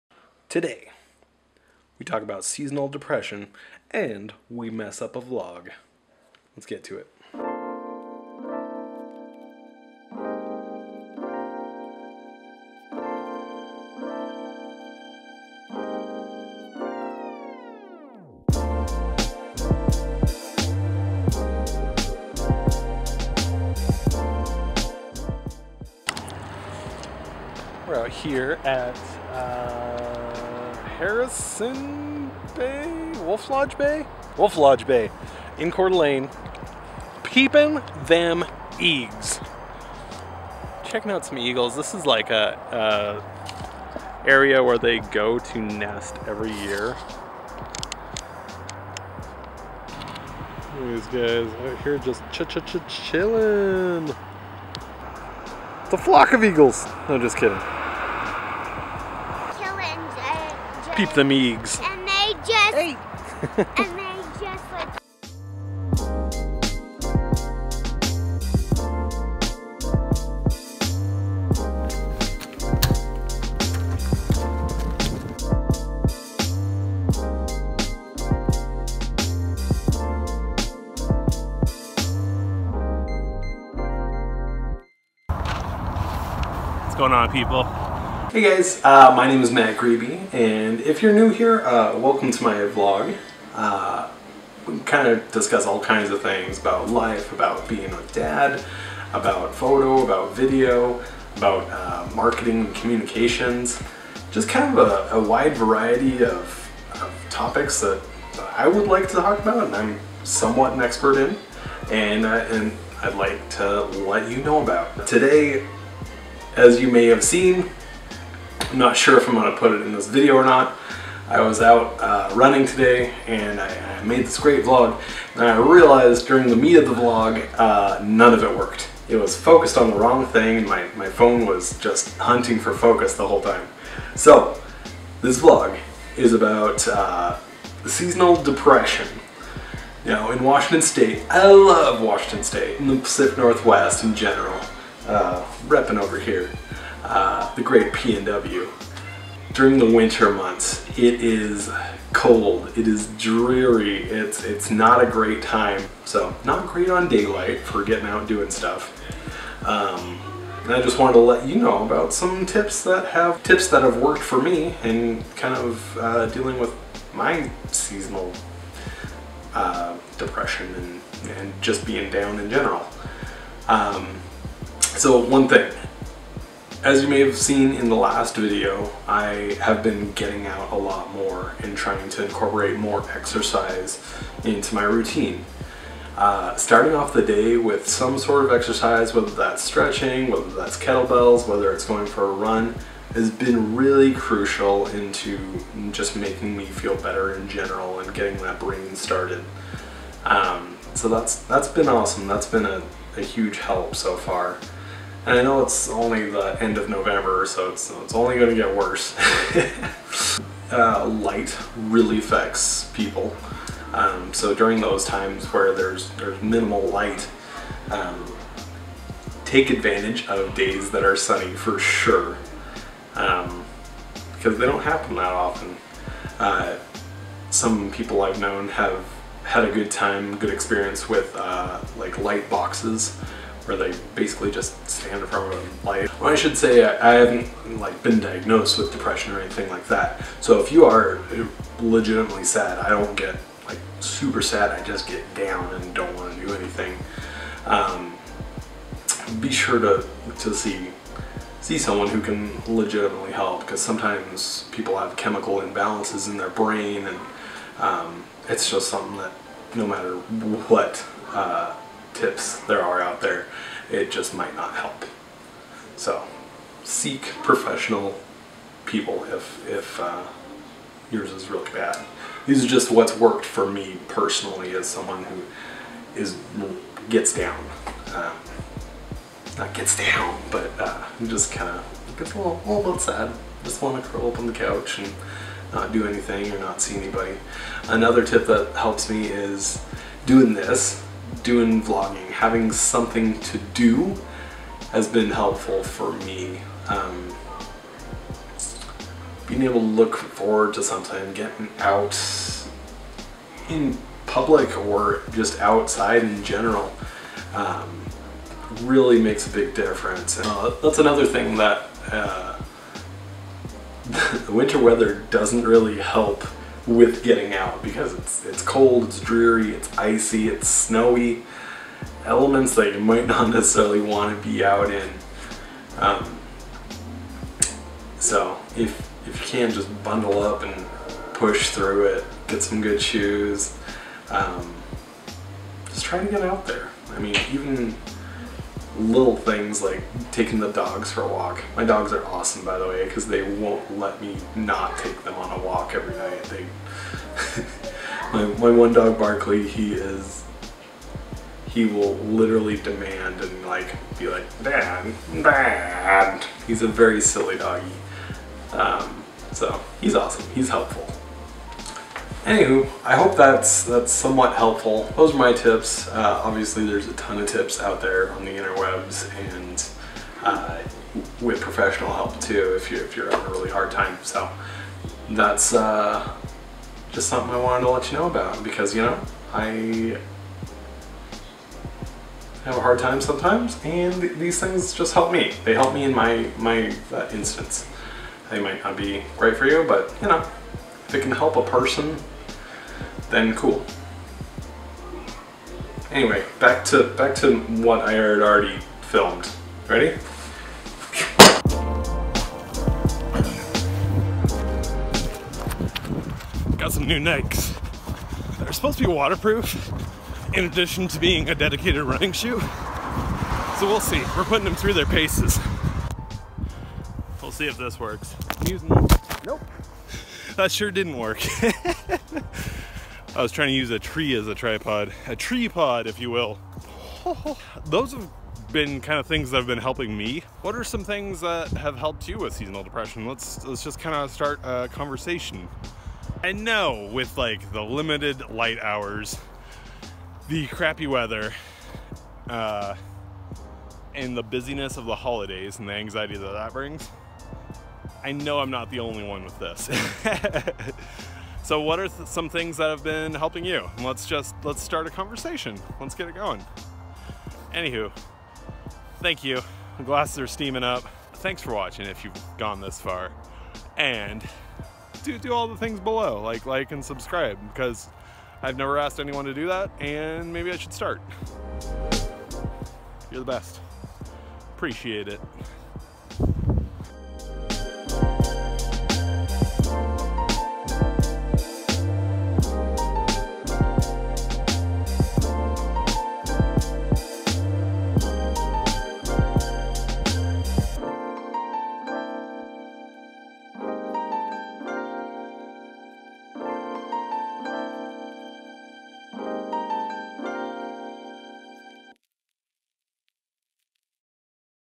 Tips to Combat the Dreary Months
In this one, we check out some eagles, winter lights and what works for me for beating the winter blues.